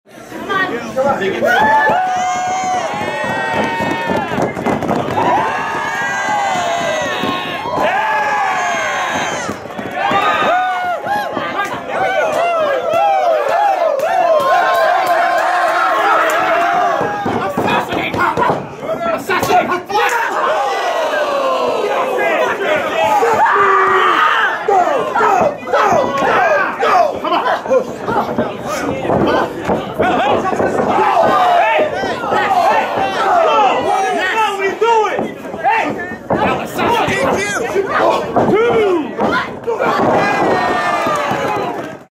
Kimber Lee Cleans House (JT Dunn & Kimber Lee vs. EYFBO) - Beyond Wrestling BONER (Intergender)
Support Beyond Wrestling:
[Preview] Kimber Lee & JT Dunn vs. EYFBO (Mike Draztik & Angel Ortiz) from the Beyond Wrestling "Battle Of New England Returns" live event. [Preview]